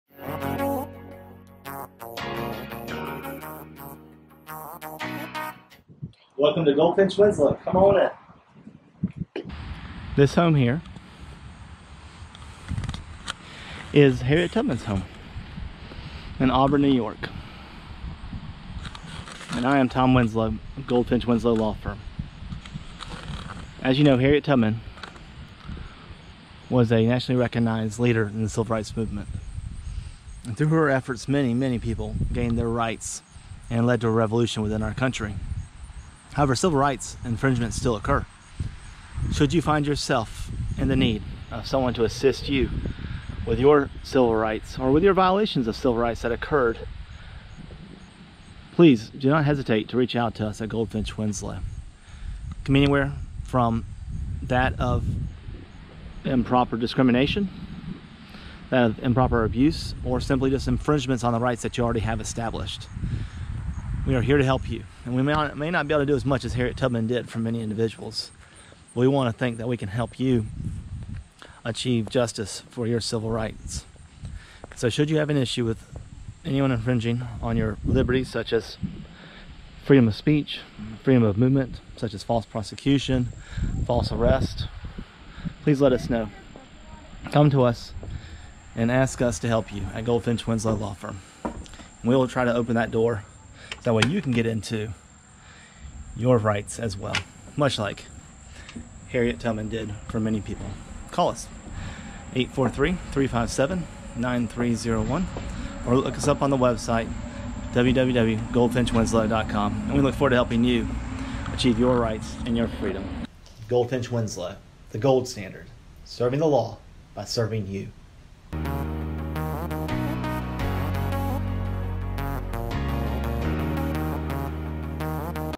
Winslow Law Vlog: February 7th
For this week's video blog, Tom discusses civil rights at the former home of Harriet Tubman.

If you have any questions or topics that you would like us to address in a future vlog, feel free to leave a comment below or contact us via a private message.